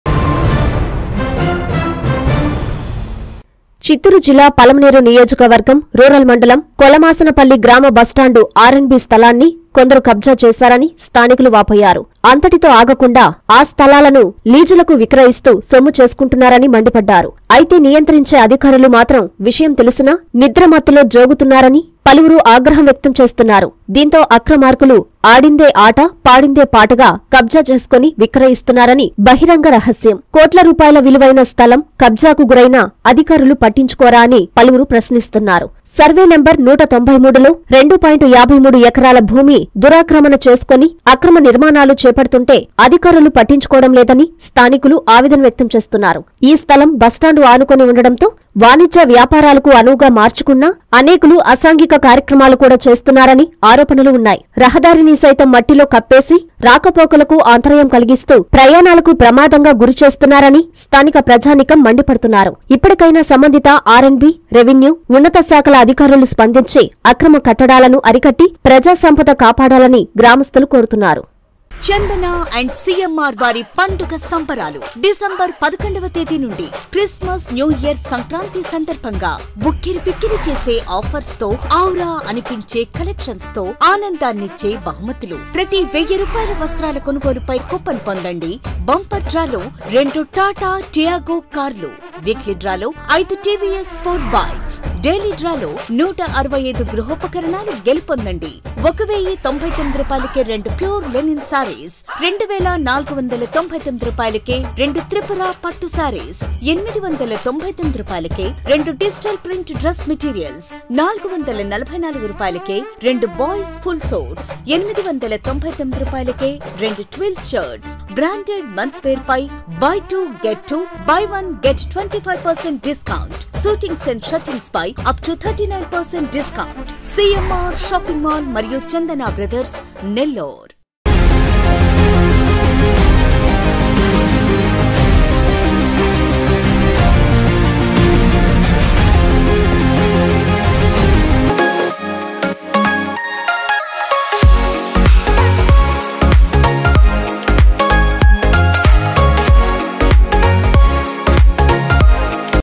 కబ్జా కోరల్లో ఆర్ అండ్ బి స్థలం
- నిద్ర‌మ‌త్తులో అధికారులు
- కోట్ల విలువైన స్థ‌లం దురాక్ర‌మ‌ణ‌
- అధికారుల తీరుపై మండిప‌డుతున్న స్థానికులు